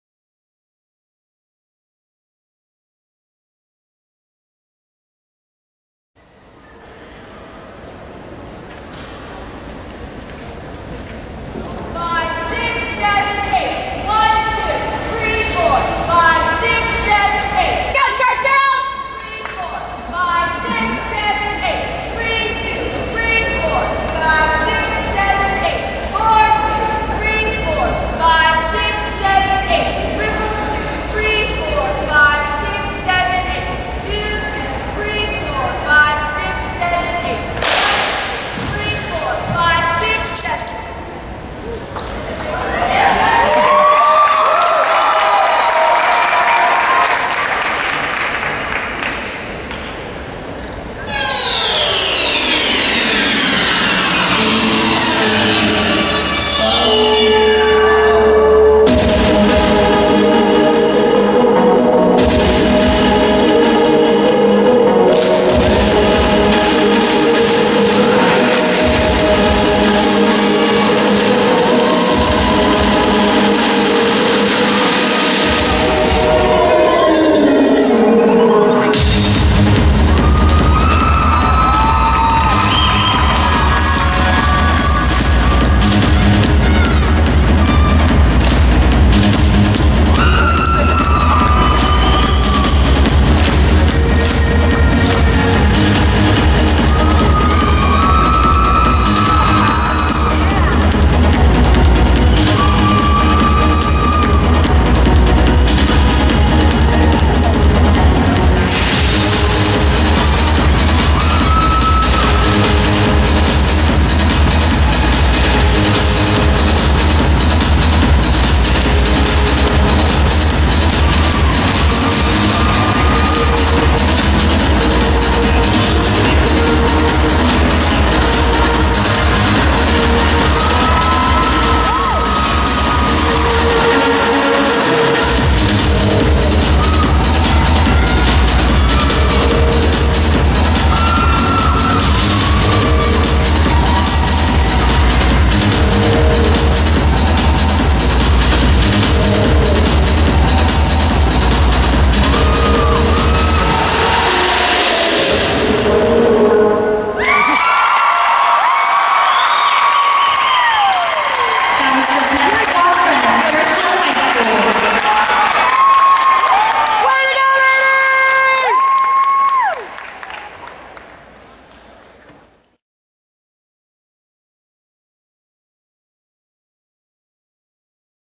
2011 - 2012 Churchill High School - Team Military Routine 02172012
An awesome military routine performed by Churchill High School at the ShowMakers Texas State Championship in San Marcos, Texas on Saturday, February 17, 2012.